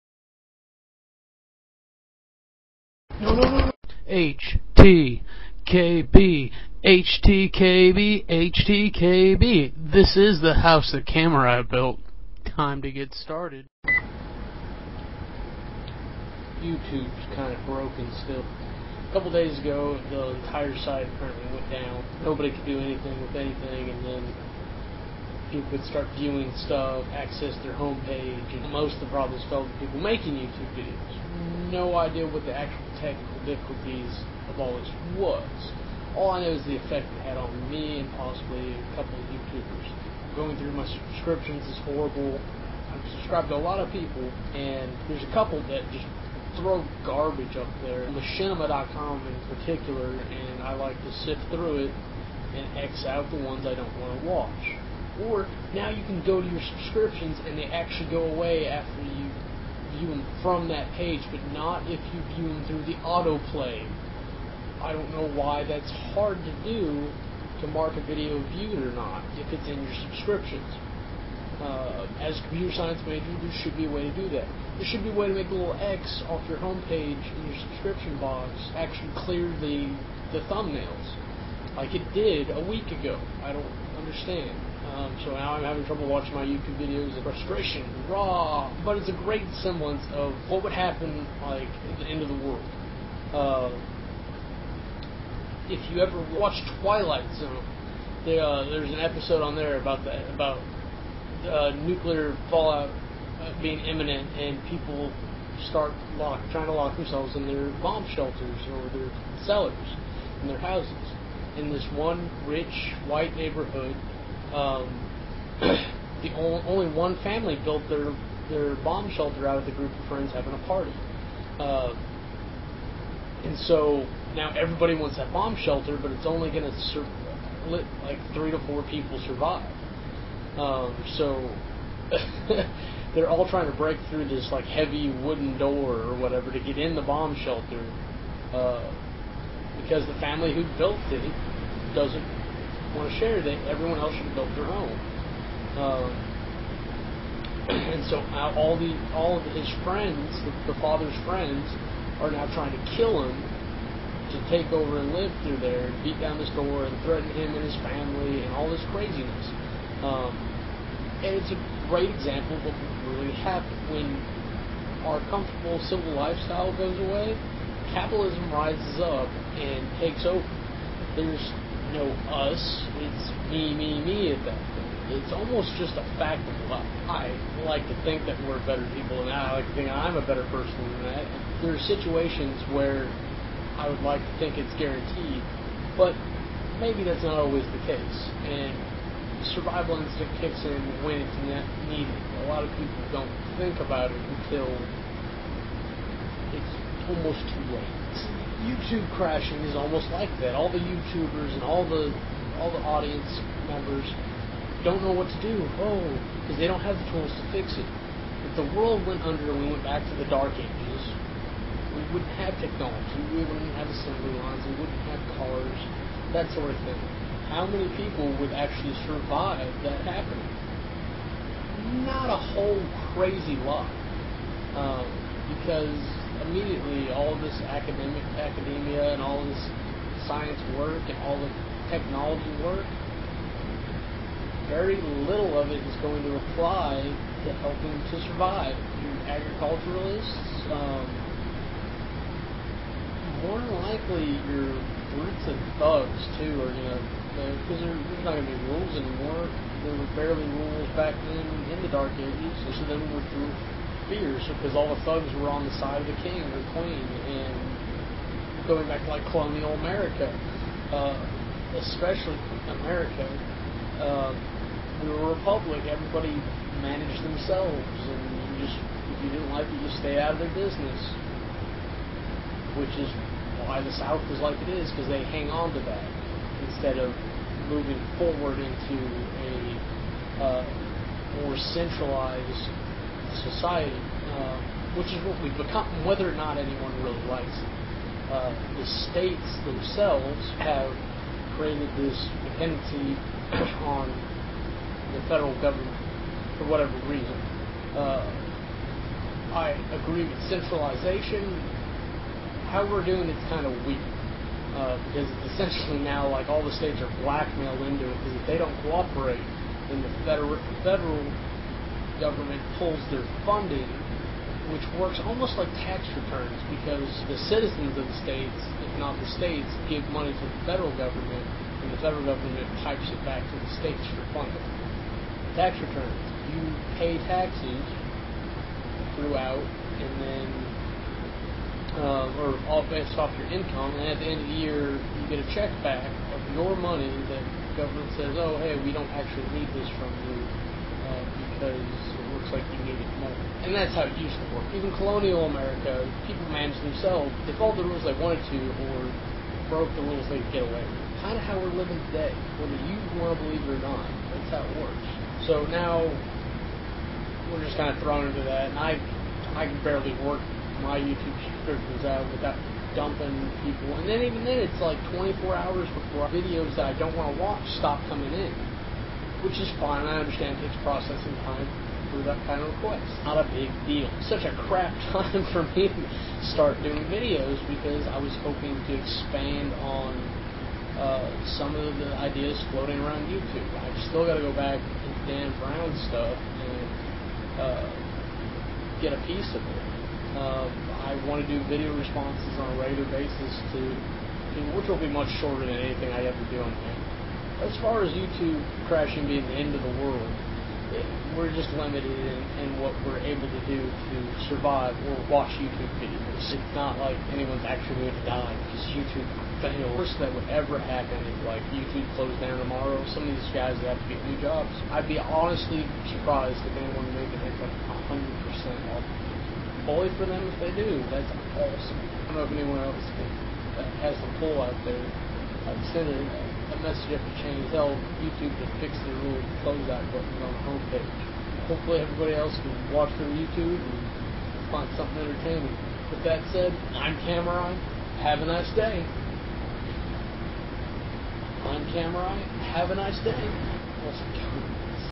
BrokenTube!
I was frustrated with Youtube being broken, and found at way to relate it to the end of the world.